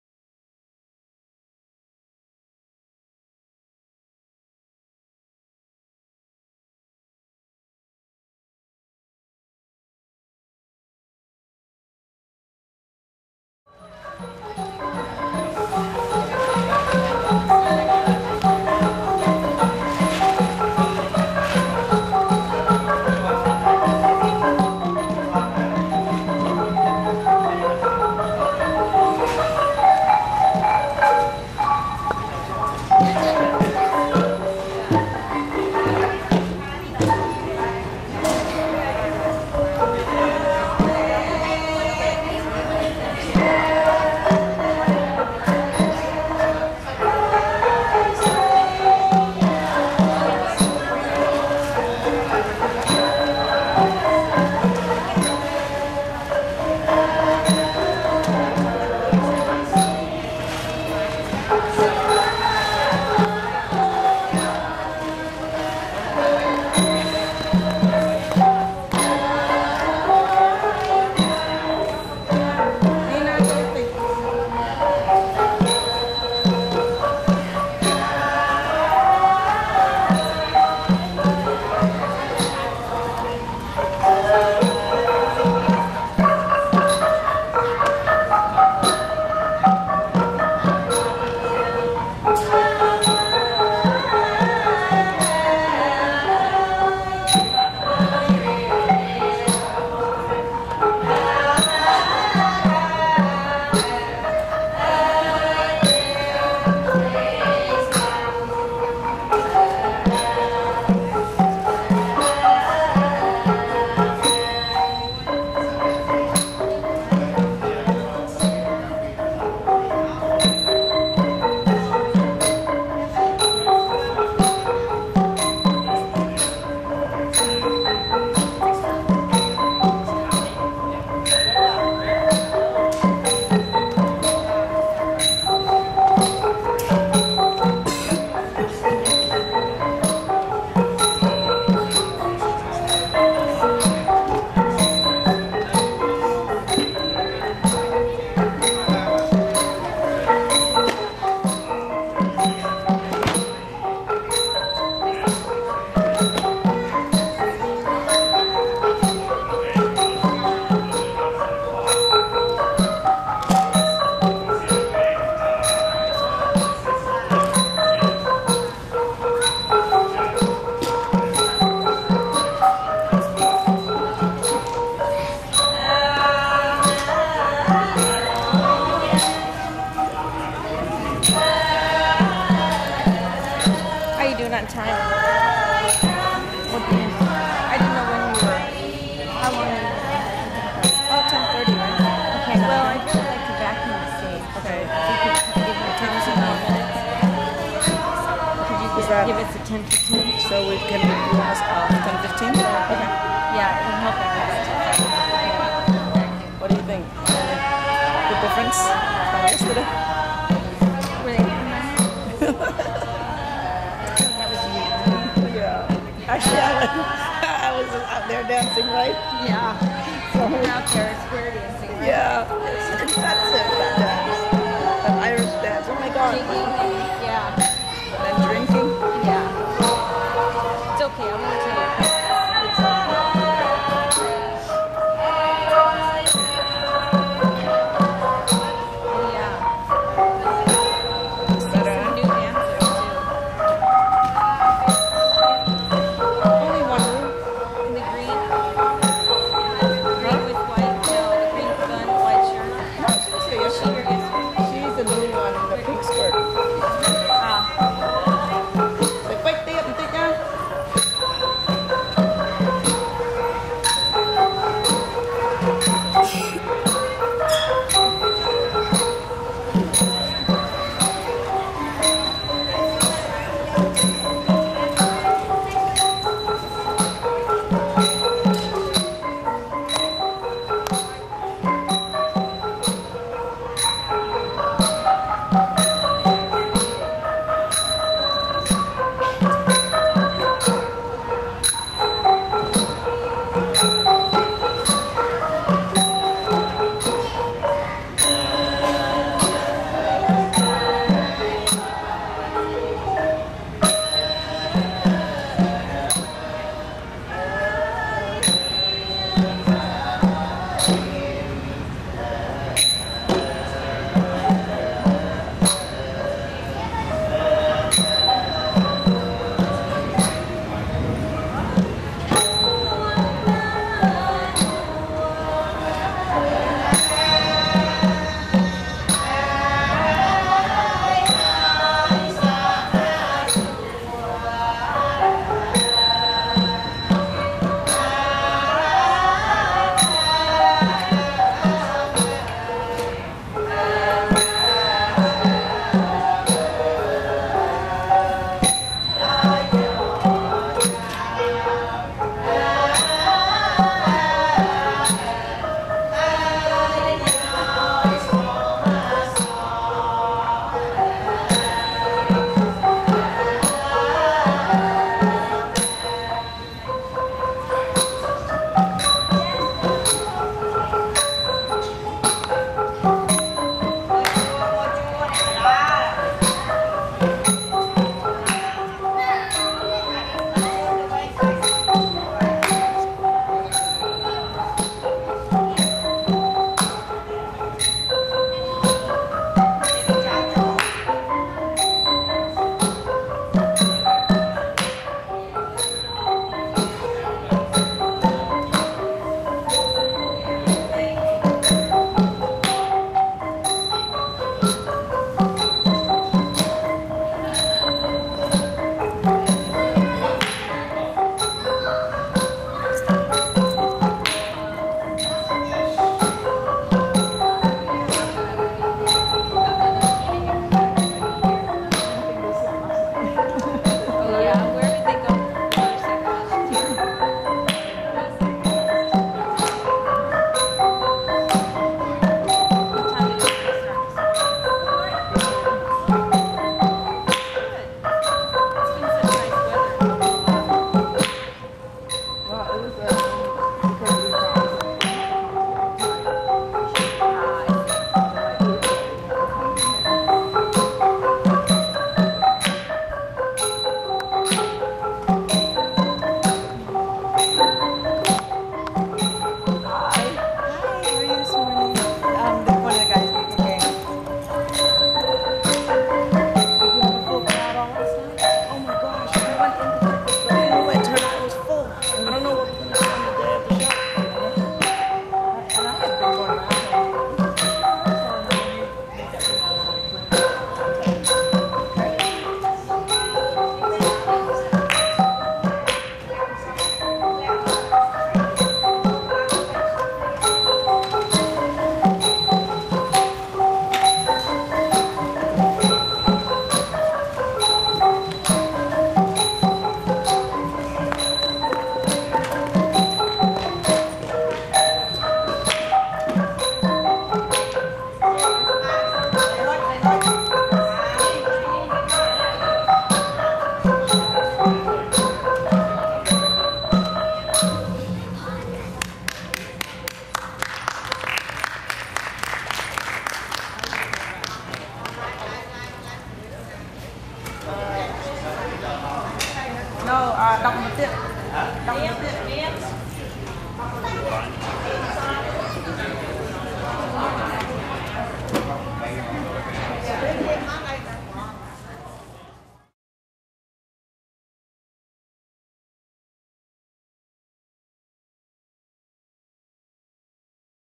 Smithsonian 2007, "Mekong River: Connecting Cultures", Danse du Cambodge II
Robam Mokor II (Danse du Makara), Répétitions, Avec les Danseuses et les Musiciens (dont Lokkru Proeung Chhieng) de l'Université Royale des Beaux-Arts du Cambodge, The Cambodian Buddhist Society Culture Group. (avec notamment Nekkru Masady Mani dans le rôle de "Voracchun"), & The Cambodian American Heritage (avec Yim Devi Dos dans le rôle de "Moni Mekkhala")...

Sans oublier, Nekkru Ros Kong, Maîtresse de Ballet...